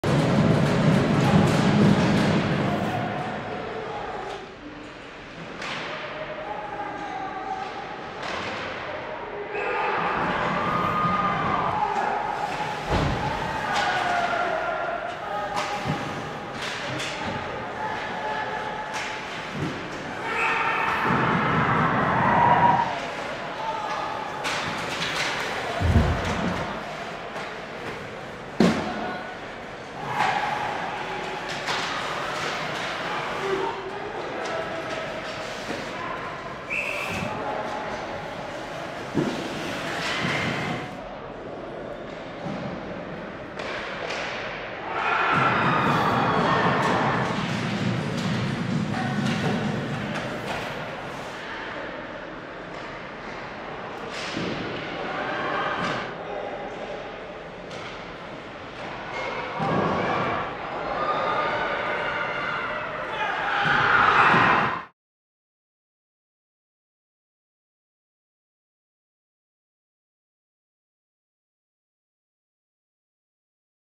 Highlights of Rangers 3-1 win over Stamps
Full game highlights of the 3-1 win by the CBHA Rangers over the NWCAA Stamps. The U18 AA regular season league game was played February 11, 2024 at the Perry Cavanagh Arena at the Max Bell Centre in Calgary, Alberta. The game was streamed LIVE on RTV.